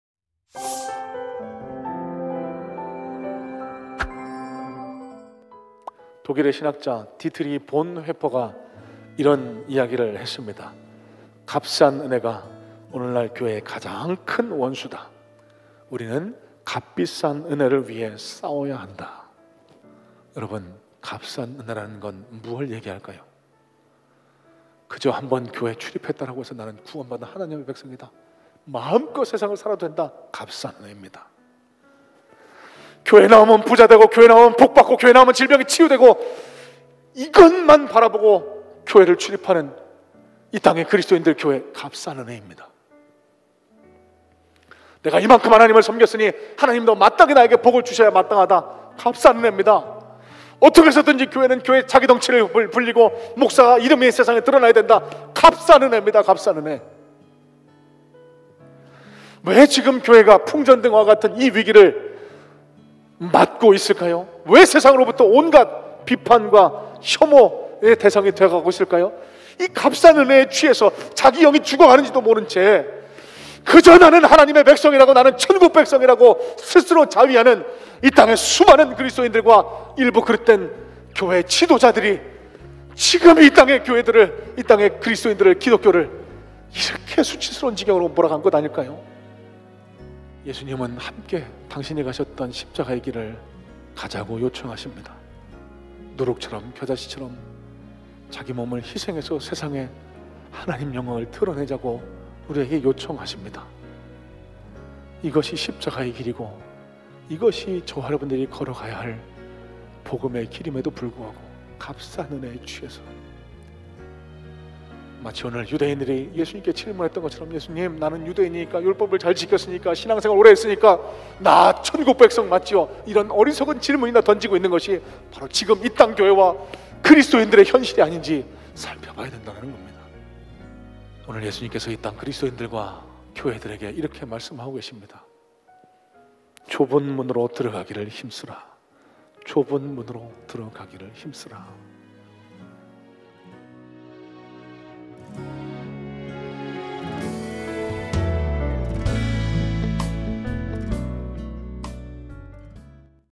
값비싼 은혜를 위해 힘써! 좁은 문으로 ㅣ 이진호목사 ㅣ 부천성문교회 ㅣ 2020-09-06ㅣ말씀돋보기
부천 성문교회 주일설교 말씀 돋보기(20.08.02)

"값비싼 은혜를 위해 힘써! 좁은 문으로"

예배 풀영상(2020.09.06 주일 3부예배 말씀 중)